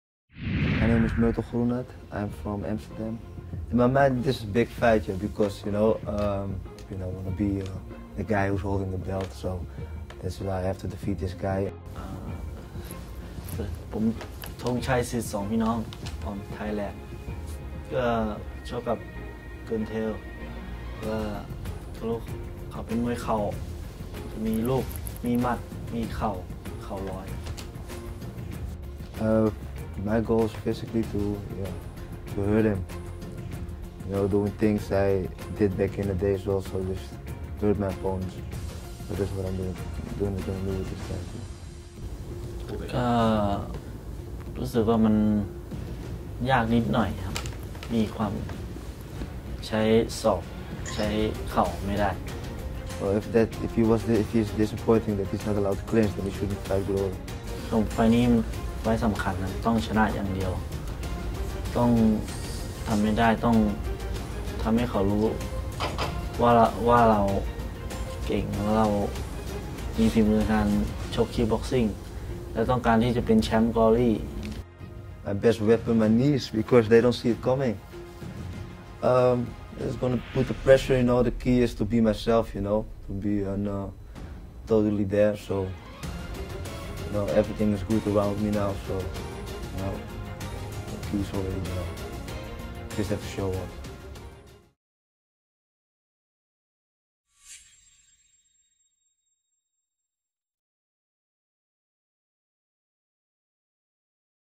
Head to Head: Thongchai and Groenhart discuss their upcoming fight
Murthel Groenhart and Thongchai Sitsongpeenong will square off in a welterweight battle this Friday at GLORY 38 CHICAGO on the SUPERFIGHT SERIES card. They are both confident of victory but only one will get his hand raised at the end.